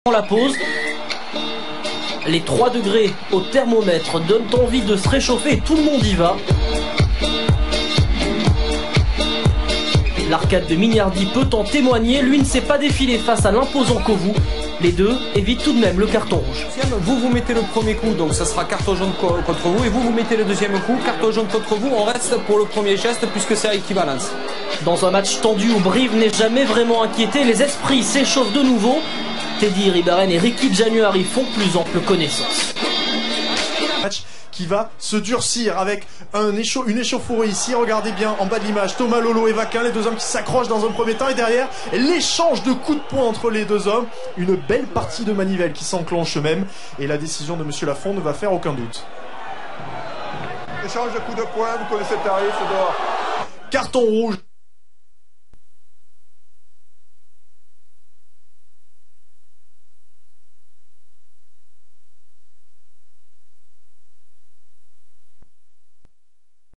Top 14 injustice et arbitage incohérent
En deux match de top 14 à Agen et à Brive des séances de coup de point qui montrent un arbitrage incohérent : 2 cartons rouges à Agen et 2 carton jaunes à Brive.Tout a commencé par un coup de poing de Setephano sur Ryan au sol. Un mauvais geste qui a échappé aux arbitres.